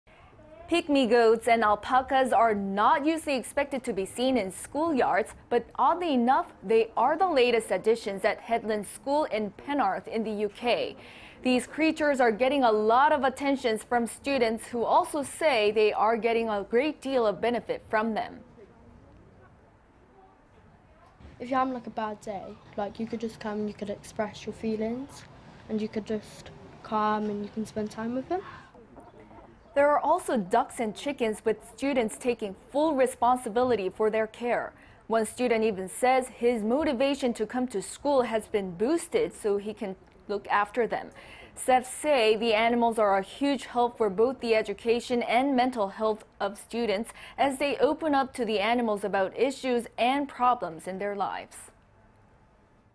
Pygmy goats and alpacas help students with their mental health
학교 운동장에 염소와 알파카들이?...영국 학교, "동물들이 학생들의 정신건강에 도움 줘"

Pygmy goats and alpacas are not usually expected to be seen in school yards,... but oddly enough, they are the latest additions at Headland School in Penarth in the UK.
These creatures are getting a lot of attention from students, who also say they are getting a great deal of benefit from them.

"If you're having a bad day you could just come, you could express your feelings and you could just calm and you can spend time with them."

There are also ducks and chickens, with students taking full responsibility for their care.
One student even says his motivation to come to school more has been boosted so he can look after them.
Staff say the animals are a huge help for both the education and mental health of students,... as they open up to the animals about issues and problems in their lives.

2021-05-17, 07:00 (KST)